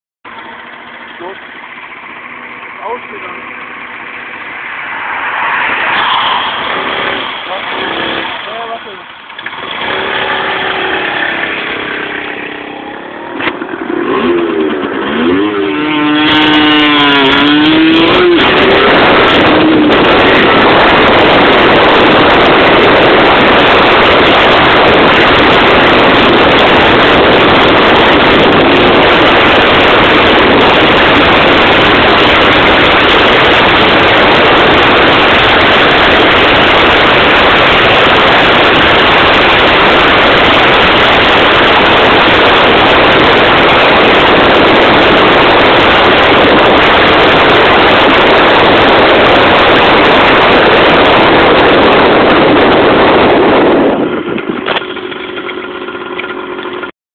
Sachs 49cm² Roller vs. Moped
Ein kleines Spaß-rennen, da wir mal wieder nichts zu tun hatten